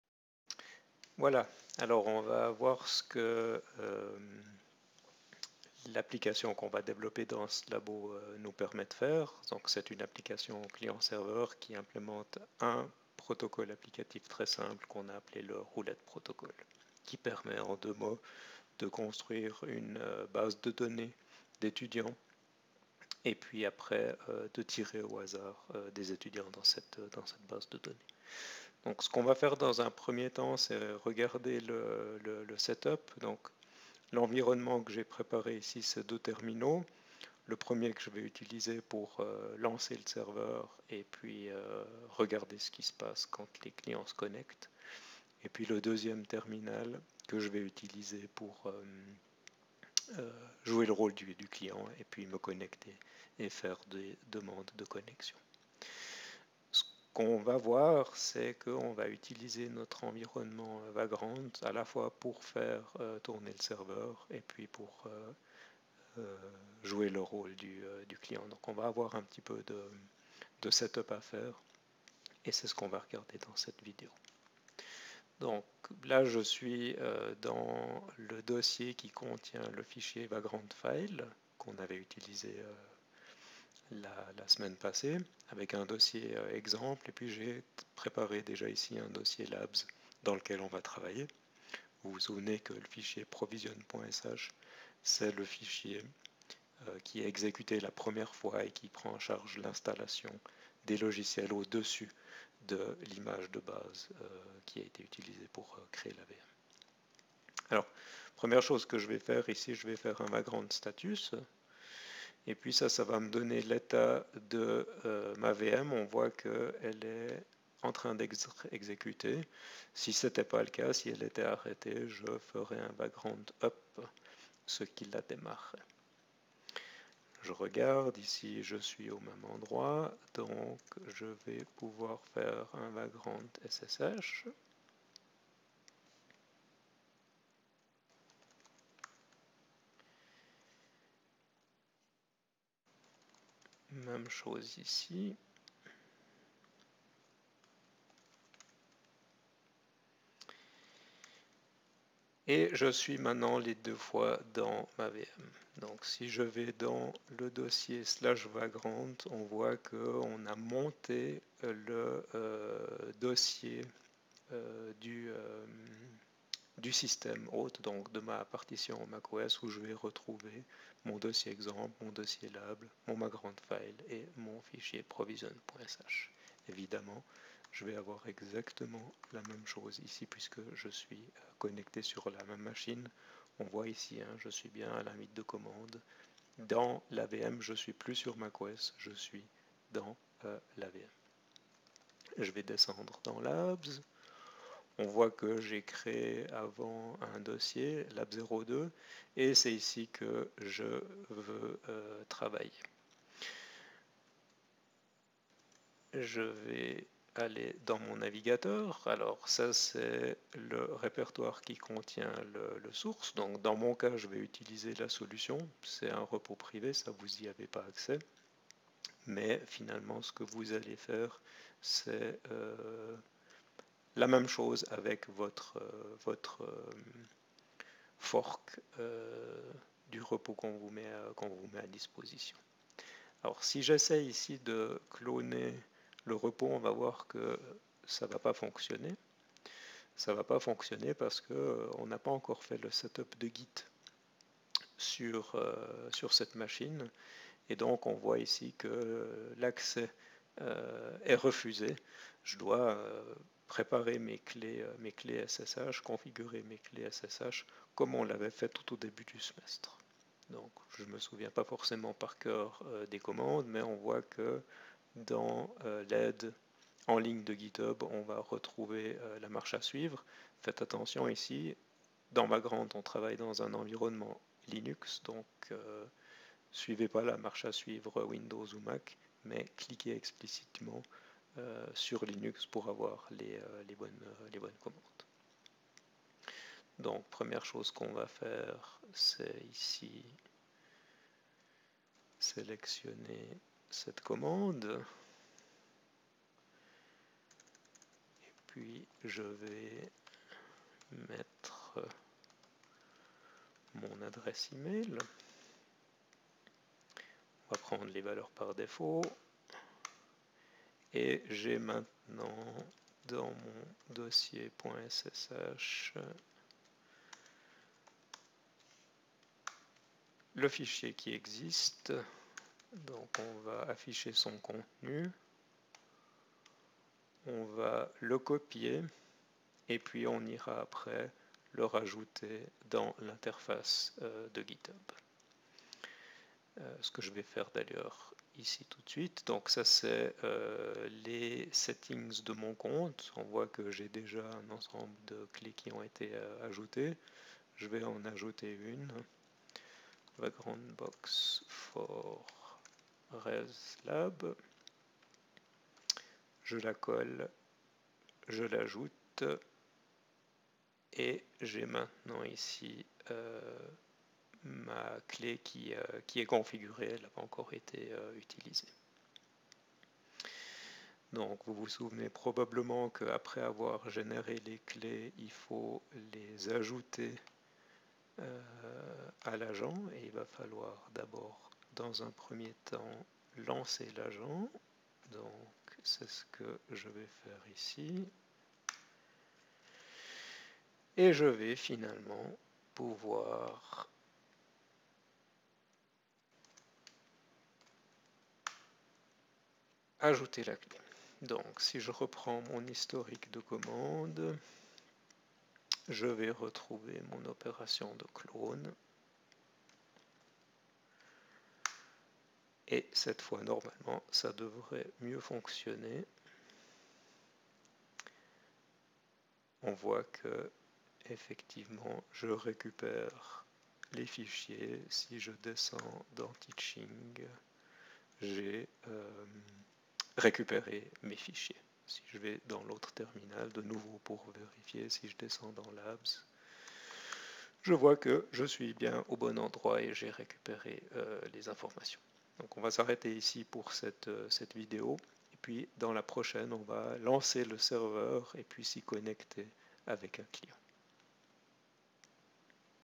RES 2015 - Lab 02 - setup
Préparation de l'environnement de travail pour le labo. Connexion à la VM via vagrant, configuration ssh pour accéder à GitHub, clone du repo.